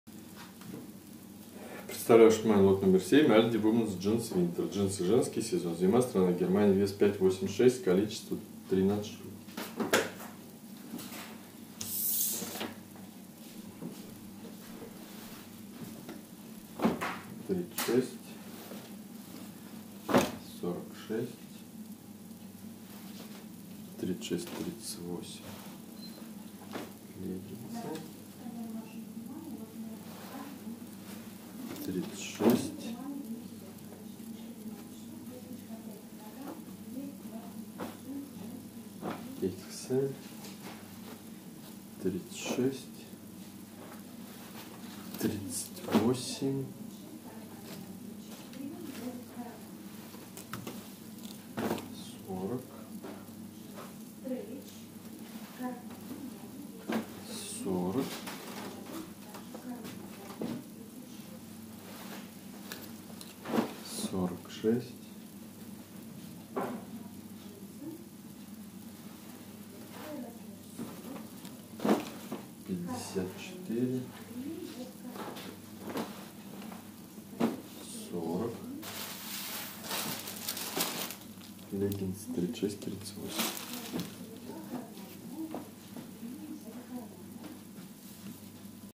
ALDI womens Jeans winter 7 ,Цена за кг : 12.9 евро ,сток одежда оптом
ALDI womens Jeans winter ( лот № 7 )
Джинсы женские  (сток)
Страна: Германия
Сезон: Зима
Цена за кг.: 12.9 евро
Цена за мешок: 75.59 евро
Цена за ед.: 5.81 евро